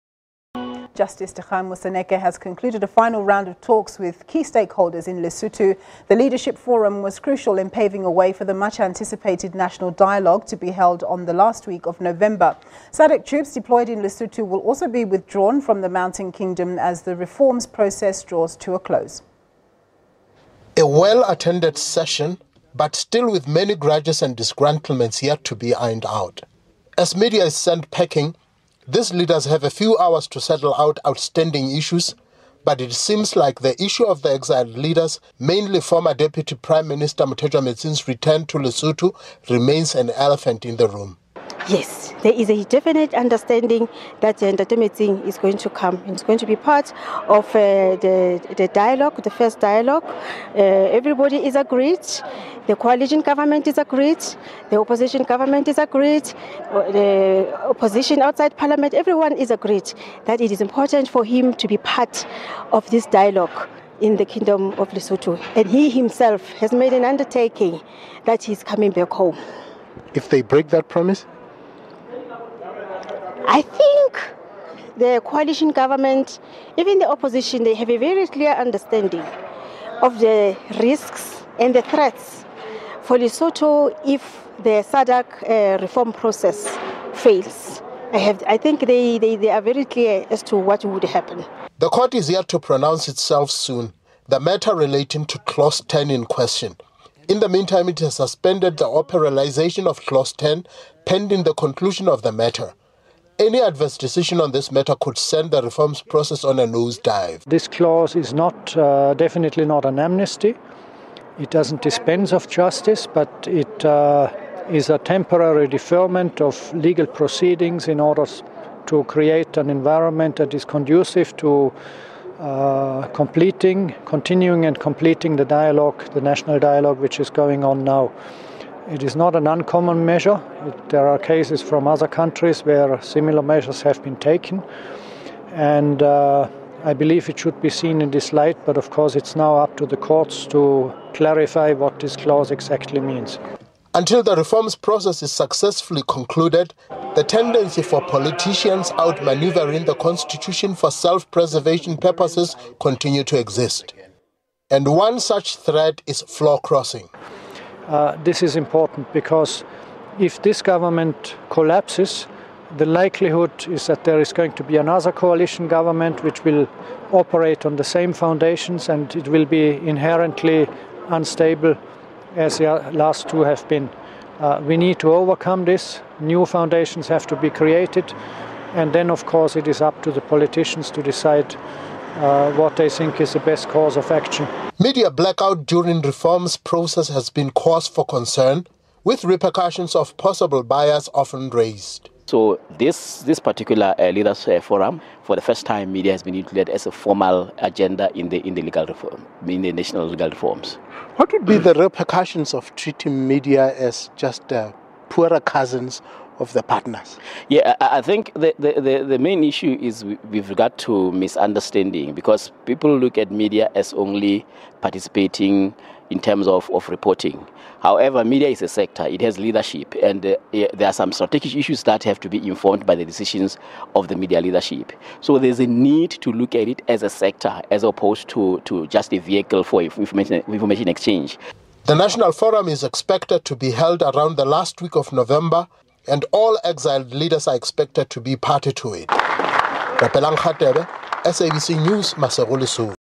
Justice Moseneke concludes talks with key stakeholders in Lesotho
Justice Dikgang Moseneke has concluded a final round of talks with key stakeholders in Lesotho. The Leadership Forum was crucial in paving a way for the much anticipated National Dialogue to be held on the last week of November. SADC troops deployed in Lesotho will also be withdrawn from the Mountain Kingdom, as the reforms process draws to a close.